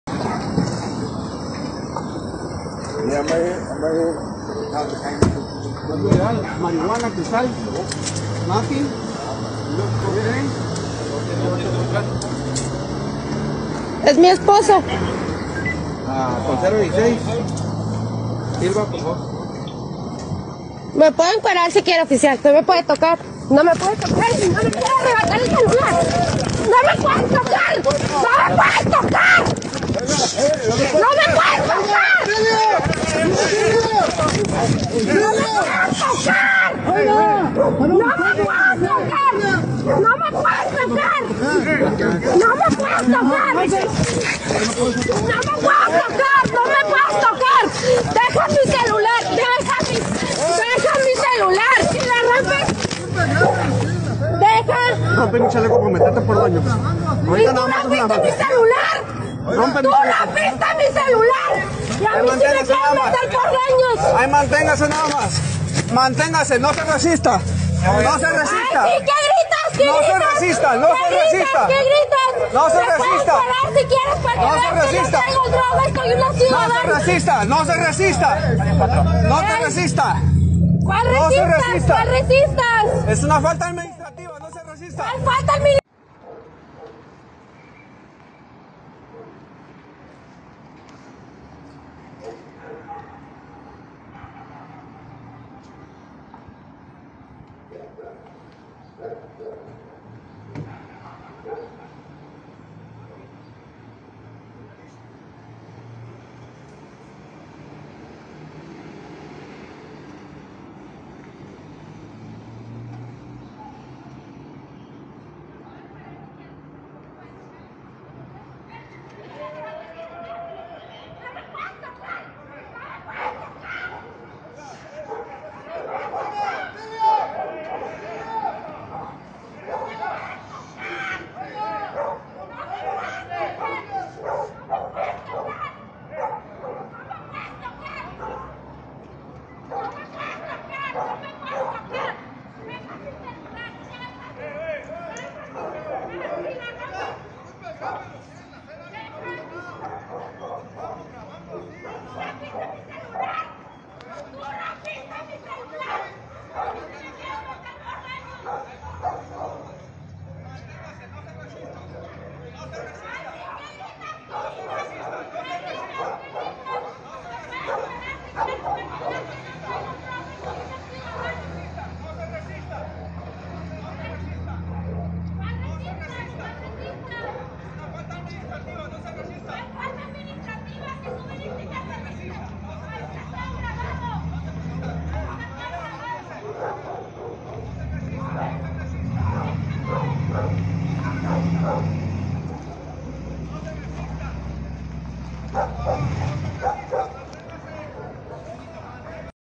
Policías de Tijuana golpean a mujer de nuevo el abuso de autoridad de los poliratotas de Tijuana🐭
Policía municipal de Tijuana somete a una mujer por grabar la intervención que le hacían a otra persona, después se acercan a ella y a su esposo, aplican candados de mano al hombre, la mujer continúa grabando y el policía se va en contra de ella.

En la grabación se escucha como la mujer grita desesperada por la actuación del elemento, mientras que éste argumenta “usted me rompió mi chaleco, es una falta administrativa”.

Ante los hechos sindicatura ha confirmado que se encuentra trabajando en el caso por abuso policial, mientras que el secretario de seguridad, Fernando Sánchez, declaró ante medios que la primera persona que revisaban los oficiales traía consigo varias cantidades de droga y que esperaría resultados de la investigación para determinar si hubo abuso policial.